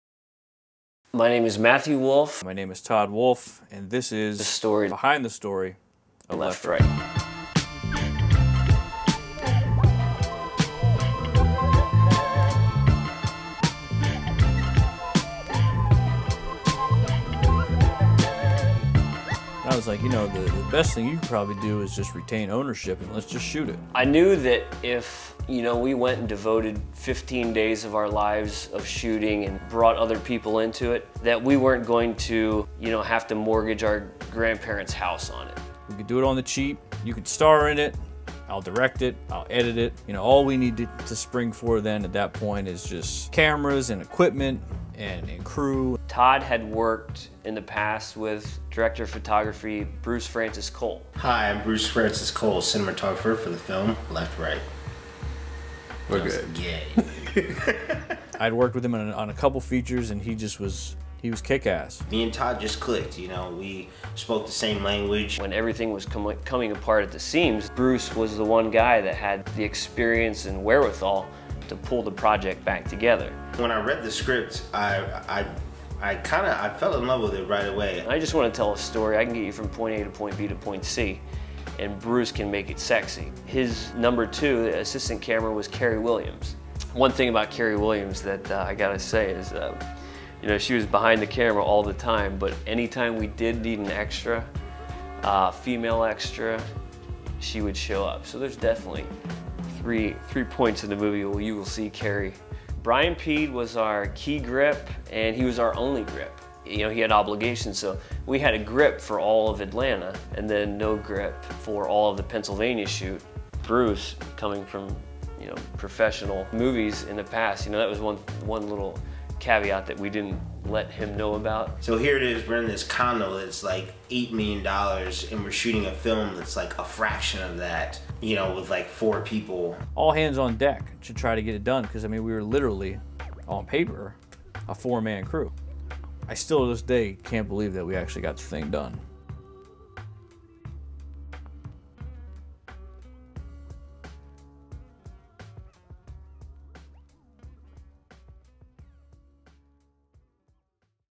Left/Right (Making Of - Part 2)
A behind the scenes look at the making of our first feature film, Left/Right. This part looks at how the Wolfe brothers geared up to shoot and features Todd Wolfe (CheesesteakMEDIA), Matthew Wolfe & cinematographer Bruce Cole.